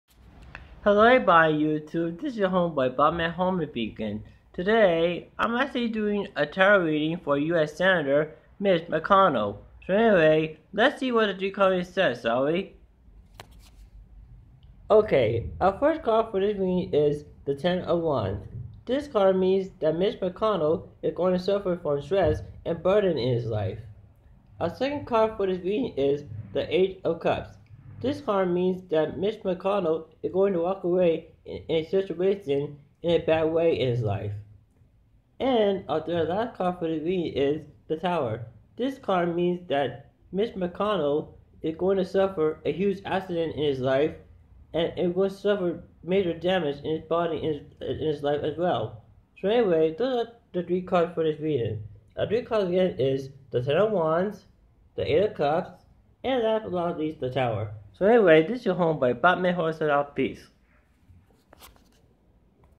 Mitch McConnell Tarot Reading
In this video, I'm doing a tarot reading for US Senator Mitch McConnell